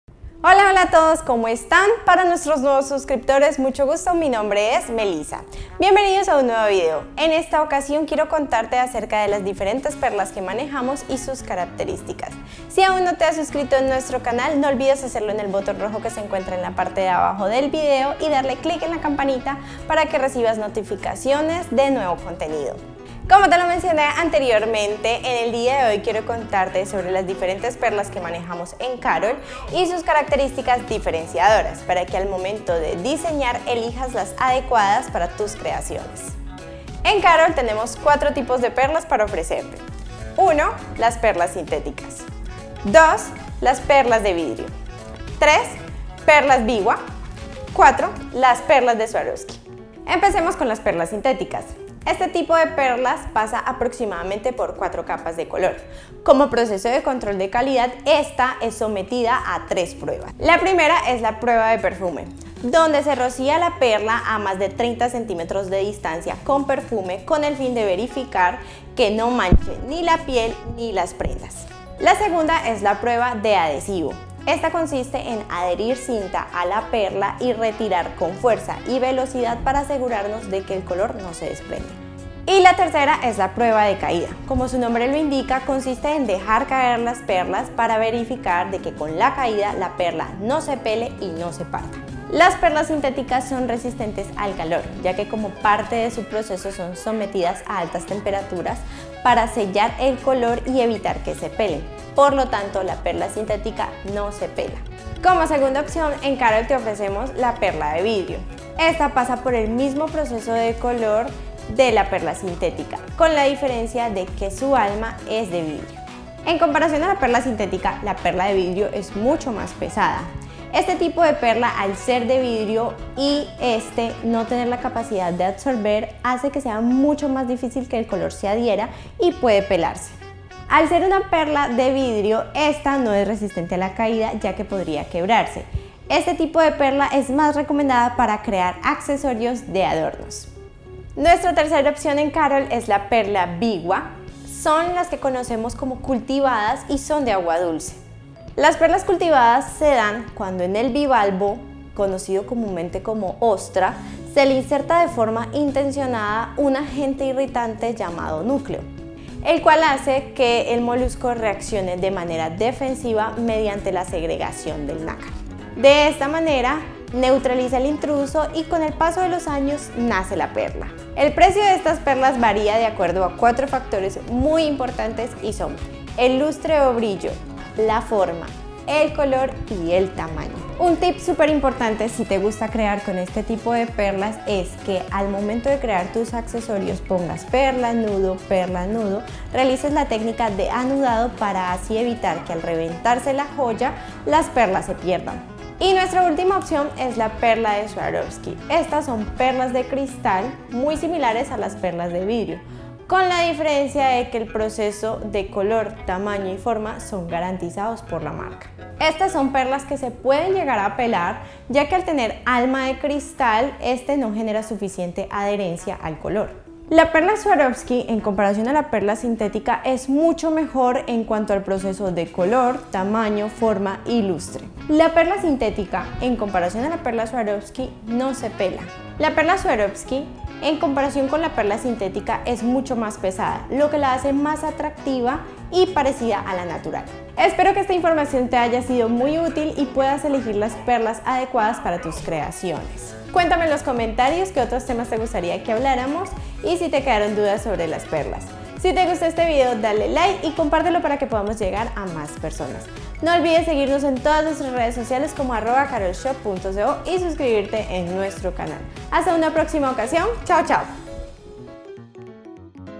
🐚PERLAS - Tipos de Perlas🐚 y sus características- Bisutería Carol
Aprende a identificar las perlas ideales para tus creaciones

Enlaces a productos y categorías

🐚Queremos enseñarte sobre las 4 tipos de perlas que tenemos para ofrecerte
🐚Descubre las características de cada uno y aprende a identificar cuales son las perlas ideales para tus creaciones
🐚Tienes alguna duda sobre las perlas
Déjame tus comentarios